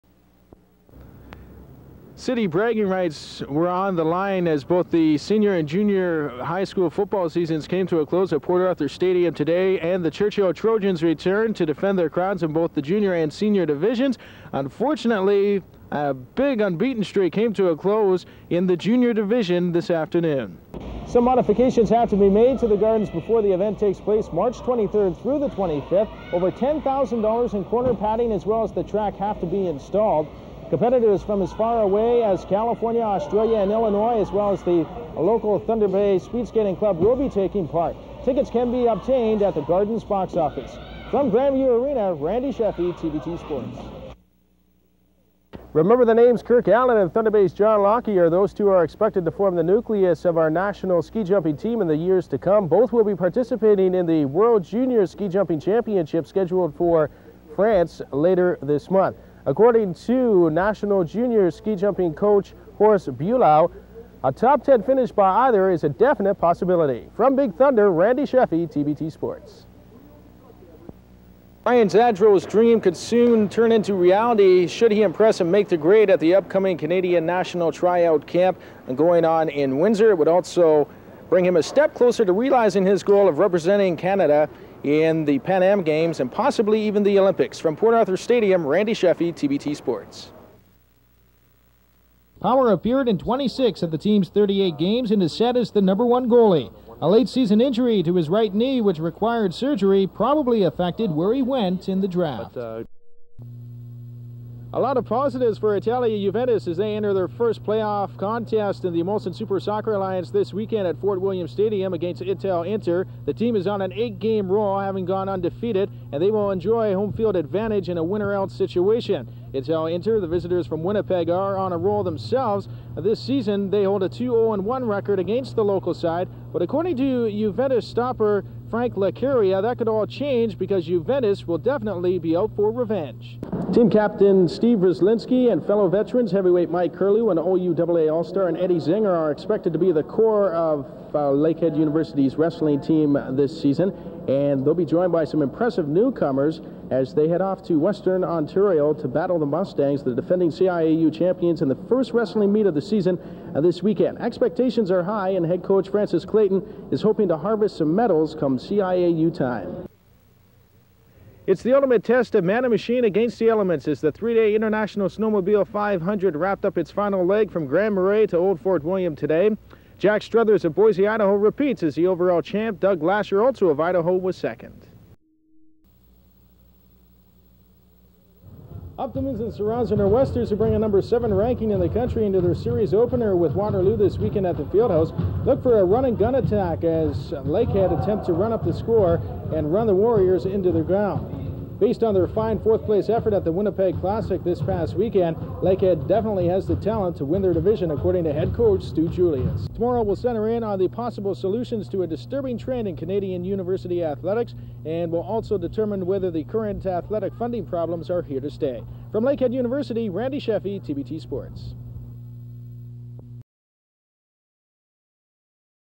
Vintage Randy Scheffee, from 1988 to 1992
Here's Thunder Bay sports legend Randy Scheffee, during his first few years at TBT Sports.  RIP Scheff